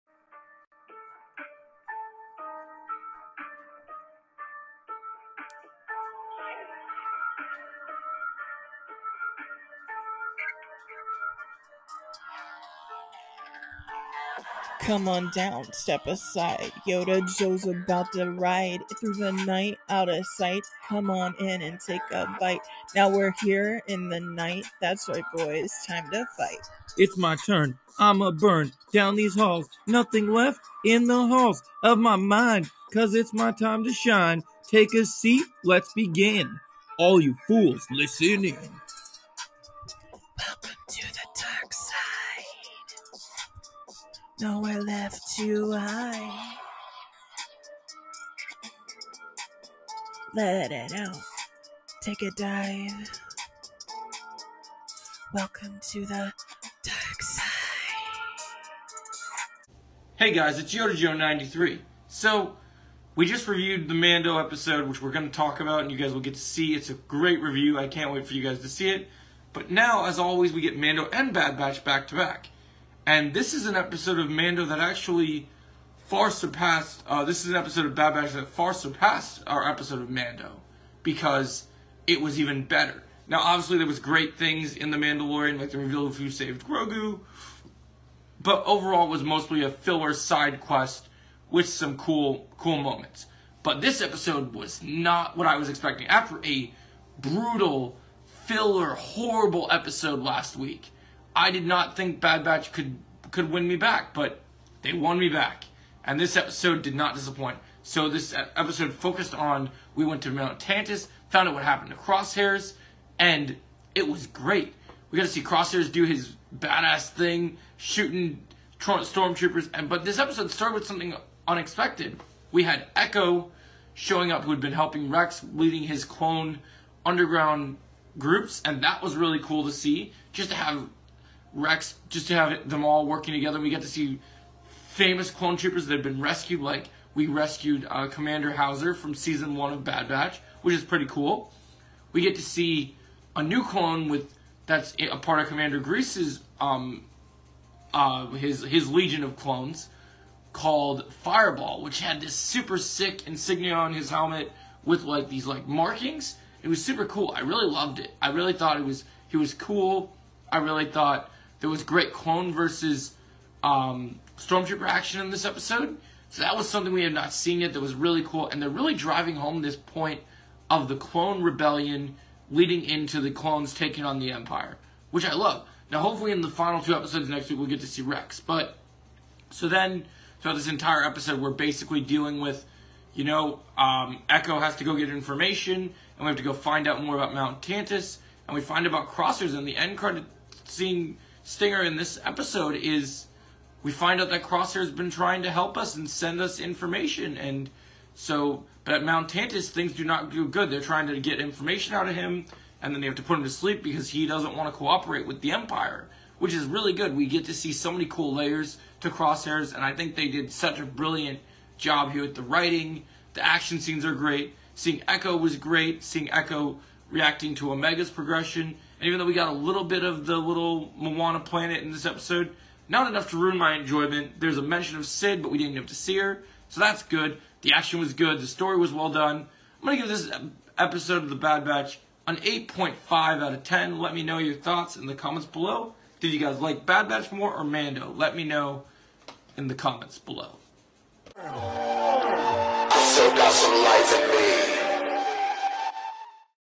Starwars Bad batch season two episode 14 review.
I meant to get this video out Earlier this week but a lot of things have come up this week it’s been very busy so things have slowed down a little bit but I hope you guys enjoy this review and I have many more videos coming for you guys today starwars lucasfilm ￼